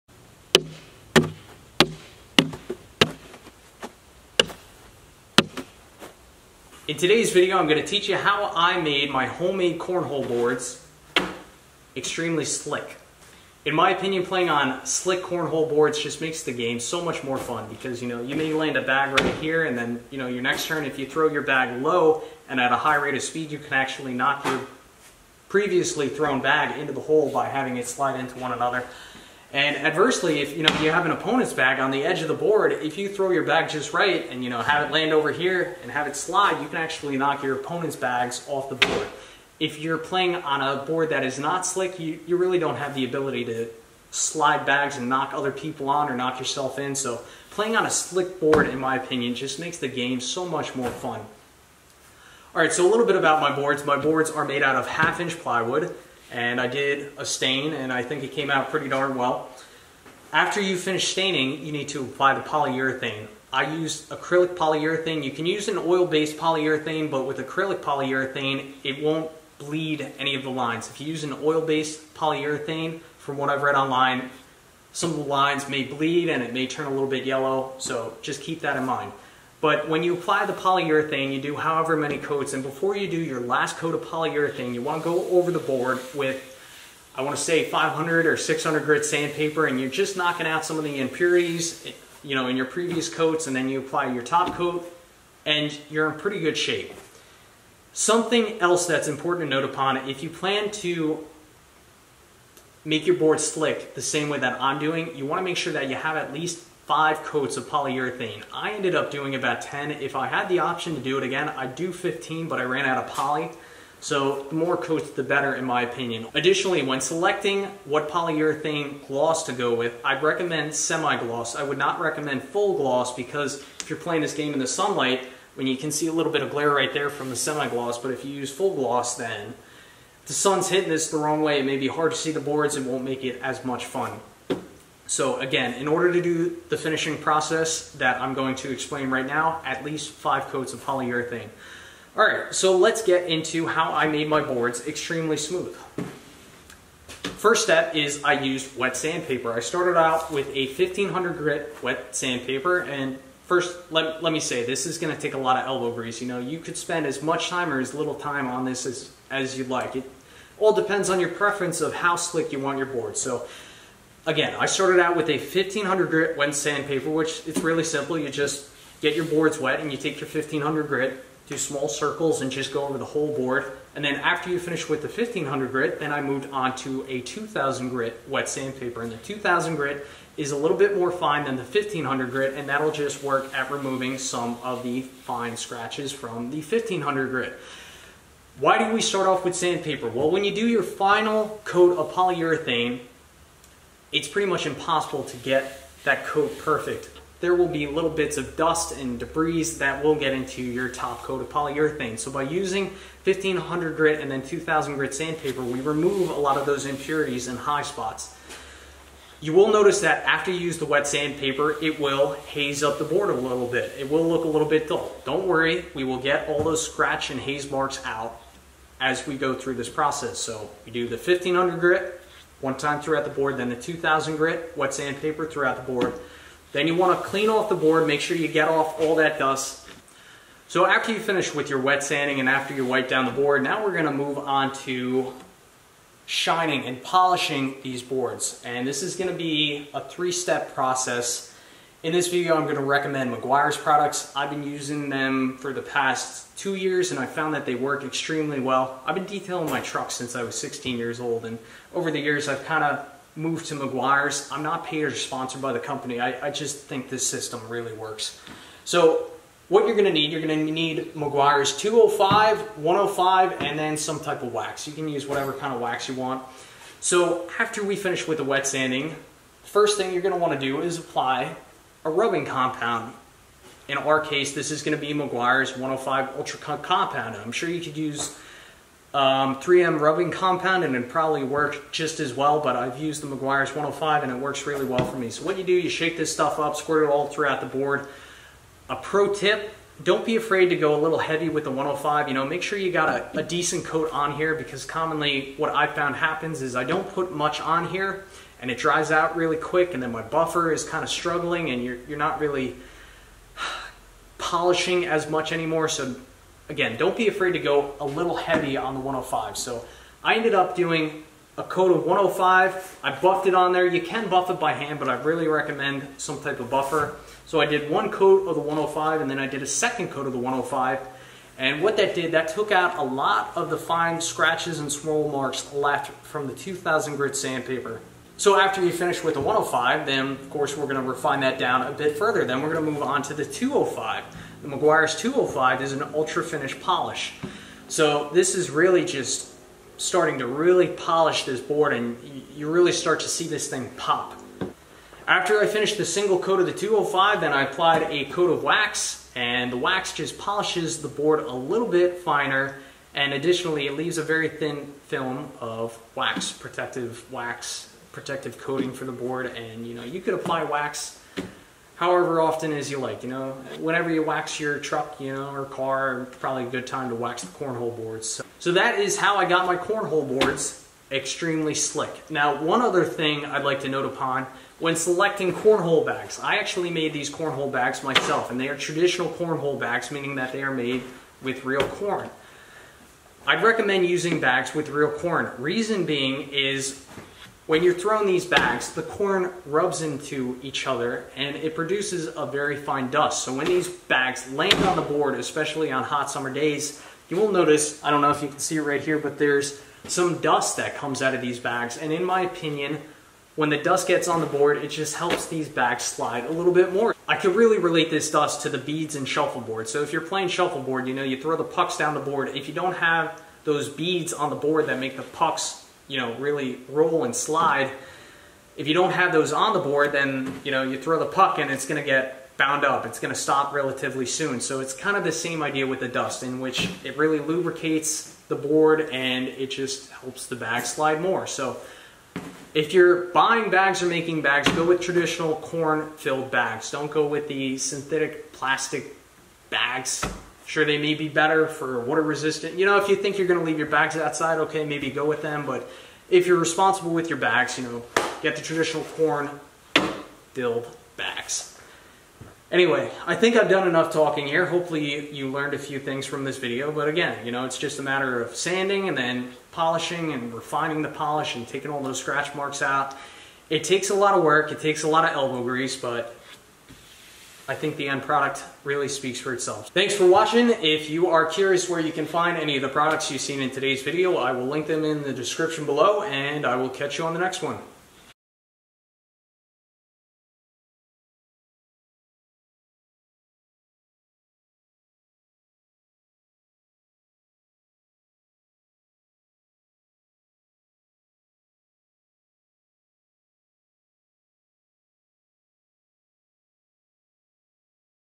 How to make cornhole boards Slick
Slick corn hole boards make the game twice as fun. In todays video, i'll take you through my 4 step process of how I make corn hole boards extremely slick.

Tools/ Products in this video

Assorted wet sandpapers

Meguiar's 105 Cut compound

Meguiar's 205 Finish/ Polish

Meguiar's Paste wax

Buffer

My Main Video Camera